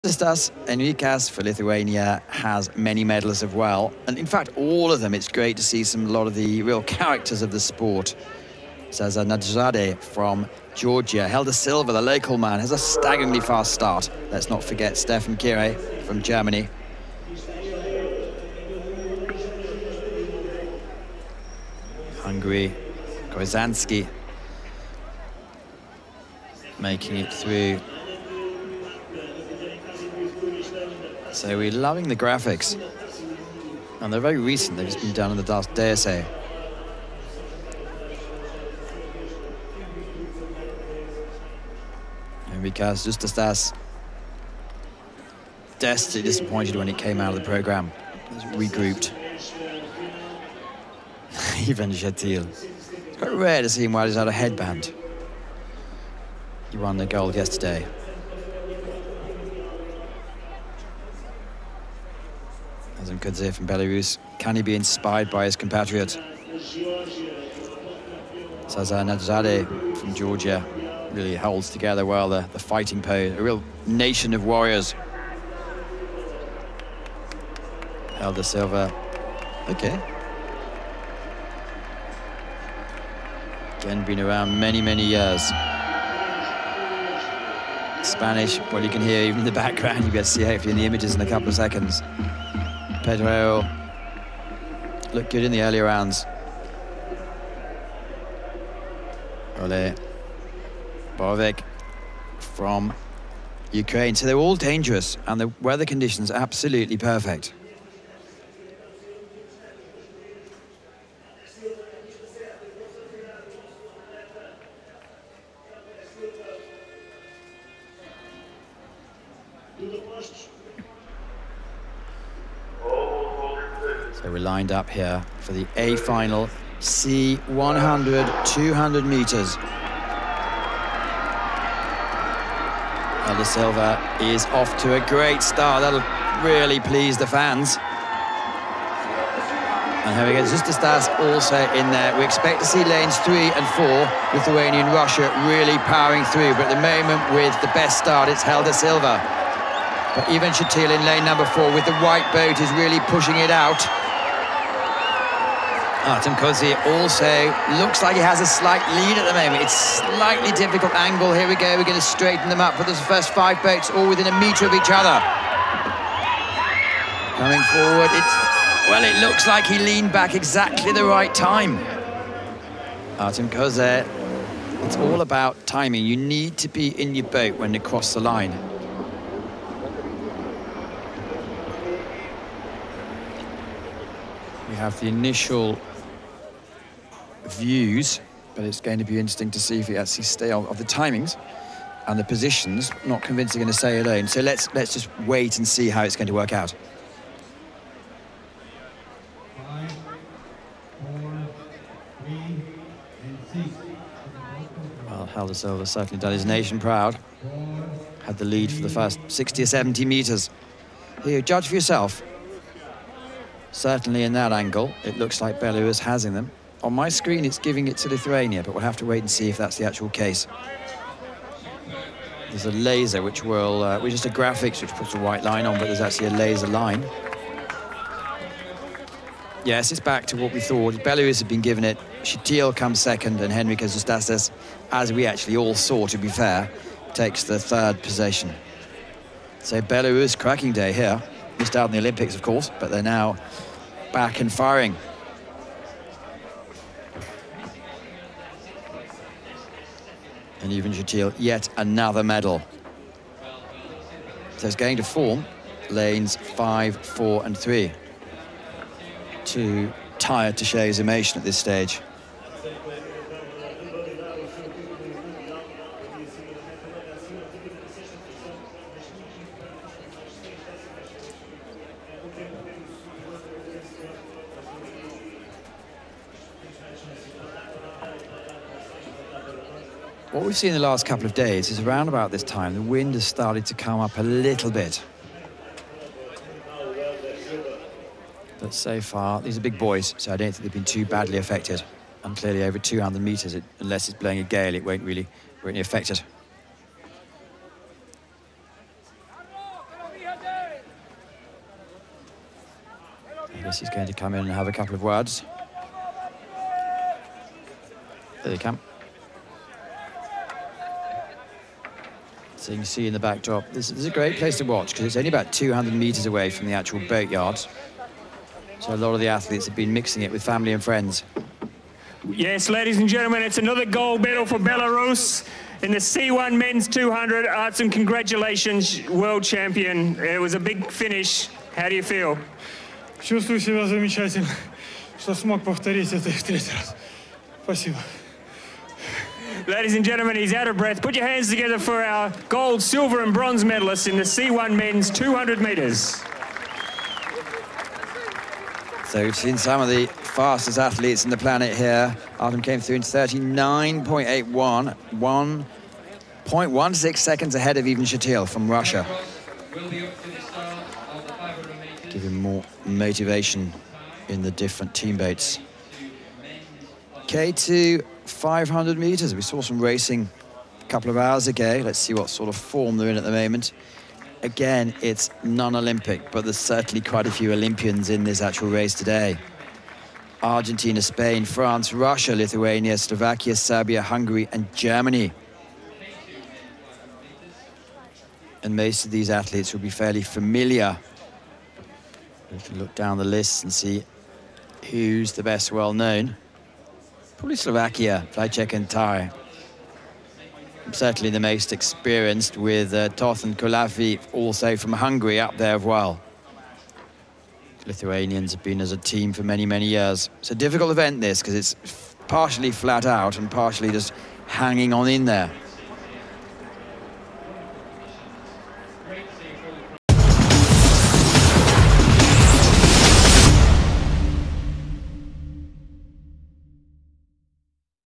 C1 Men 200m Final / 2018 ICF Canoe Sprint World Championships Montemor
Revisit the ICF Canoe Sprint World Championships held on 22–26 August 2018 in Montemor O Velho Portugal